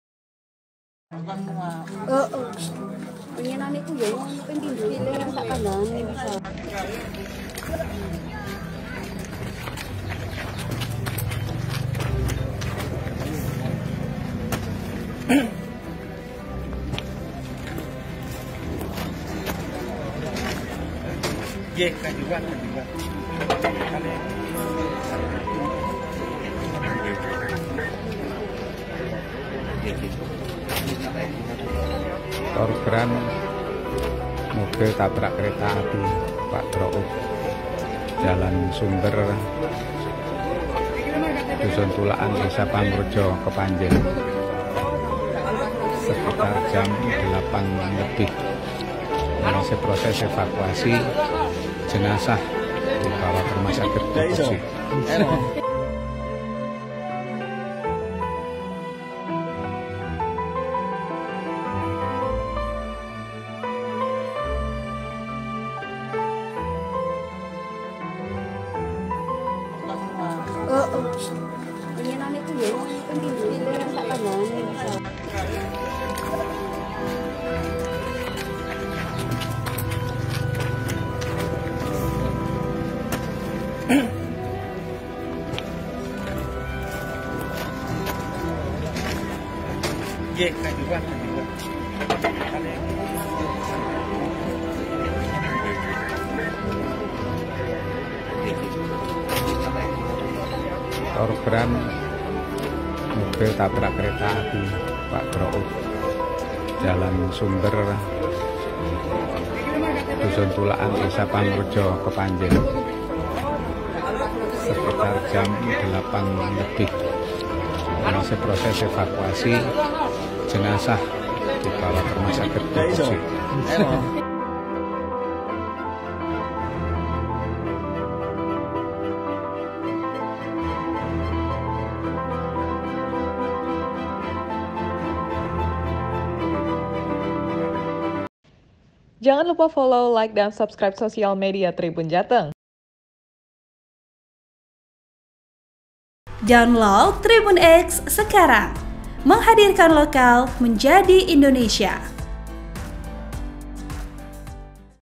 Kecelakaan Odong-odong Tertabrak KA Penataran Terseret 100 Meter, 1 Meninggal
Terjadi kecelakaan KA vs odong-odong di perlintasan KA sebidang dengan palang pintu di Desa Panggungrejo, Kecamatan Kepanjen.

Akibatnya, Liswanto (58) warga setempat yang merupakan sopir odong-odong meninggal dunia di tempat kejadian perkara.

Peristiwa ini terjadi sekira pukul 08.00 WIB.

Sedangkan sekira pukul 11.45 WIB odong-odong baru saja diangkat dengan mobil derek.

Tupi, warga setempat yang tinggal tak jauh dari lokasi kejadian mengaku kaget saat mendengar suara tabrakan tersebut.

Editor : Yga